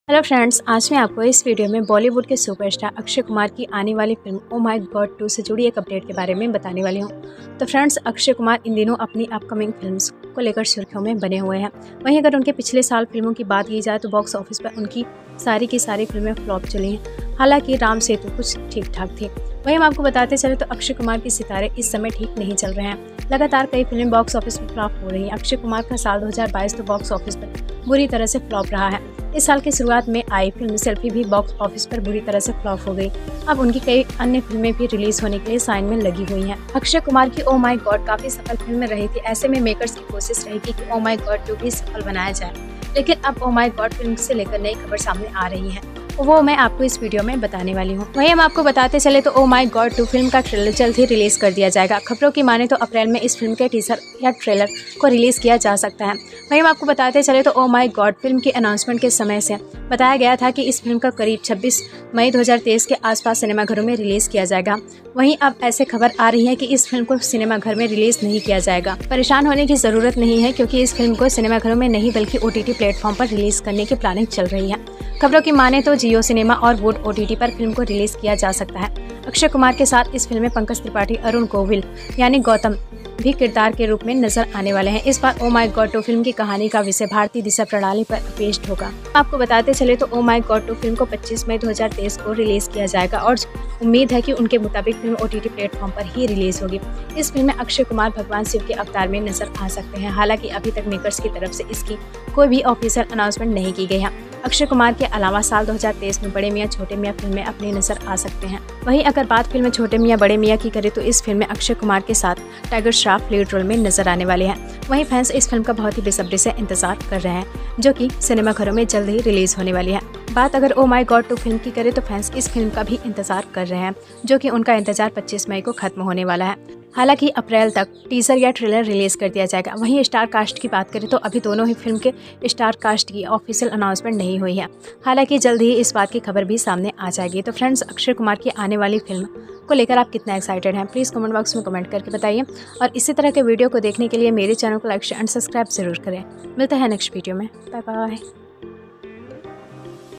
OMG 2 OTT release, Back to Back flop se dare Akshay Kumar....
OMG 2: Parde per nahi hogi release, Back to Back flop se dare Akshay Kumar.